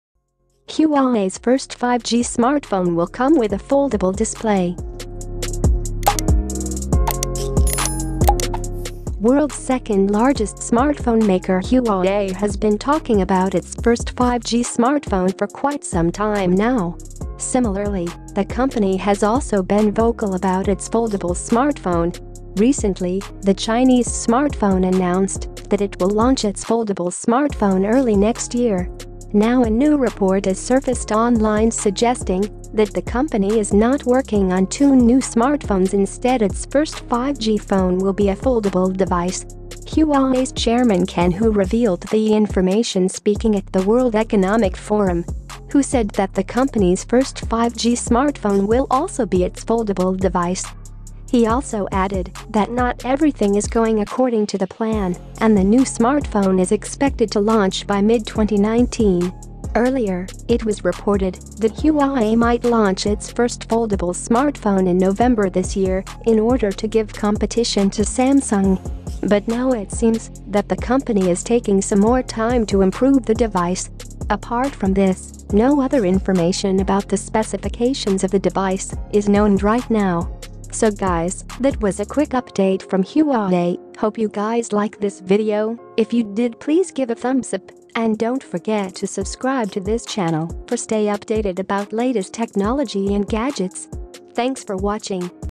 Huawei’s First 5G Smartphone Will Come With A Foldable Display!
World’s second largest smartphone maker Huawei has been talking about its first 5G smartphone for quite sometime now. Similarly, the company has also been vocal about its foldable smartphone. Recently, the Chinese smartphone announced that it will launch its foldable smartphone early next year.